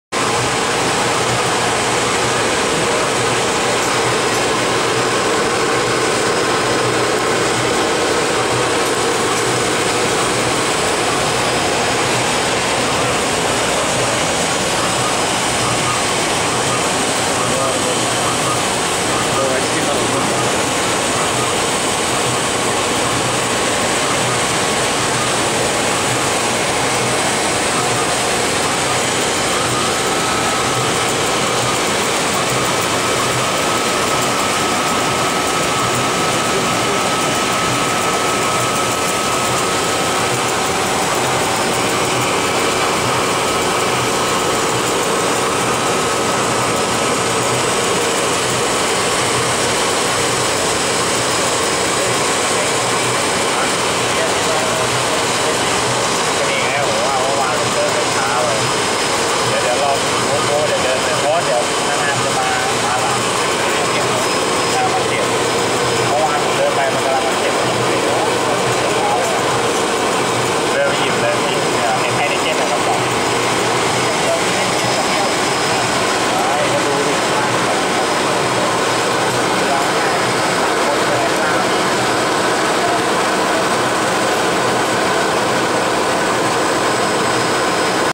Ultra Micro Lab Blown Film Line - Labtech Engineering
Lab Laboratory extruder, blown film line.  World's smallest blown film line, fully functional, with horizontal extruder.  Now offered as a production built unit.